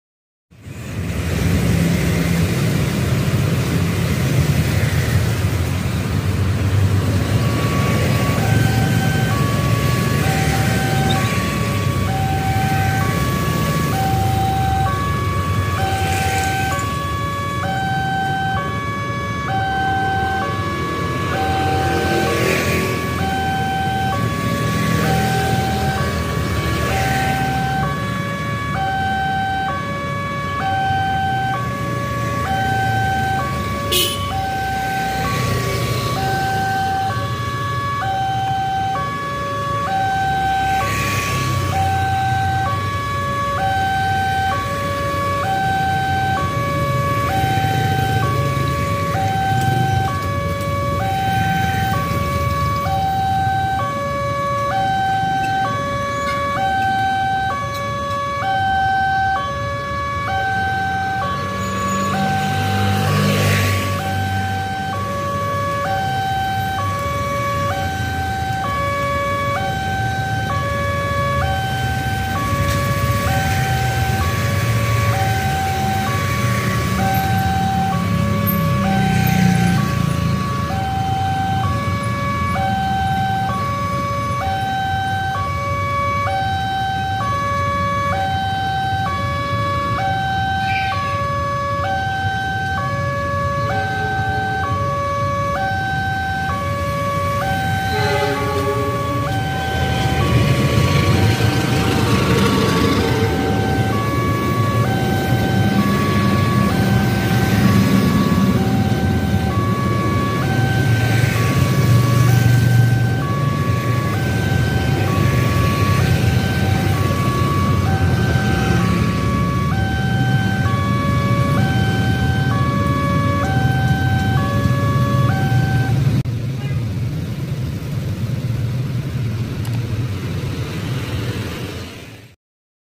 Ka Kamandaka Pagi | Nonton Kereta Api di Perlintasan JPL 215 Ar Hakim Tegal
Palang Perlintasan Kereta Api JPL 215 Ar Hakim Tegal Nonton Ka Kamandaka Pagi dengan Locomotive CC 206 menuju Stasiun Tegal